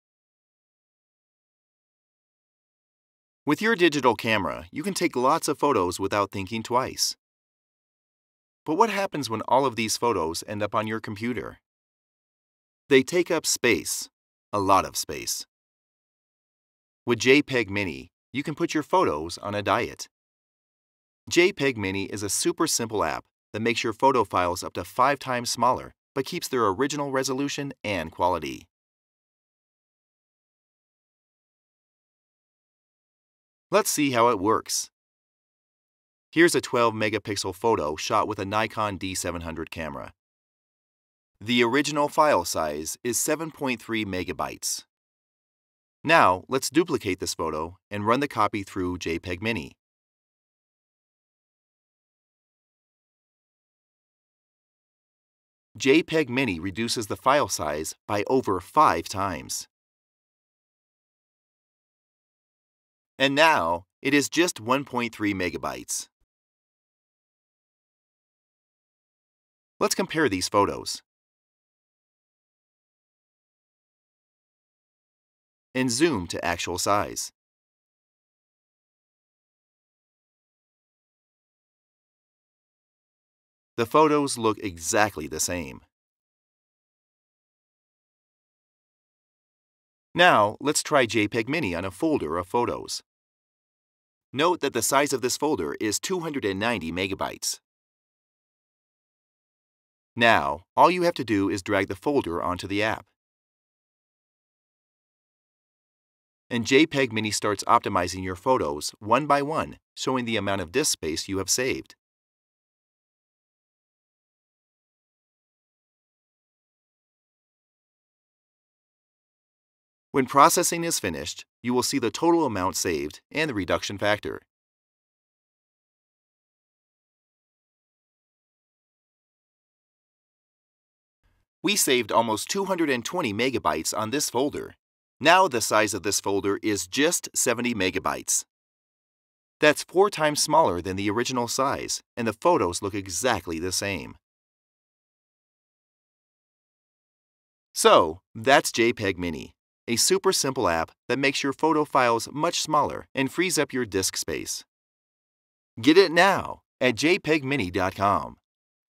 JPEGmini - Your Photos on a Diet!
Based on patented image optimization technology, JPEGmini is an app for Mac and Windows which reduces the file size of your photos by up to 5x, while keeping their original resolution, quality and JPEG format. With JPEGmini you can free up valuable hard drive space on your computer's internal and external drives (especially important for those expensive SSD drives), just by dragging your photo folders onto the app.  After JPEGmini optimization, you'll be able to attach dozens of full resolution photos to email messages, store up to five times more photos on Dropox or other online services, and back up your photo collection much faster.  You can also use JPEGmini to optimize web images and thumbnails, making your website or blog load faster.

JPEGmini supports photos with a resolution of up to 28 Megapixels, and works on Mac OS X 10.6.8 or higher, Windows XP SP3, Windows Vista, Windows 7 and Windows 8.

So what are you waiting for?  and put your photos on the JPEGmini diet today!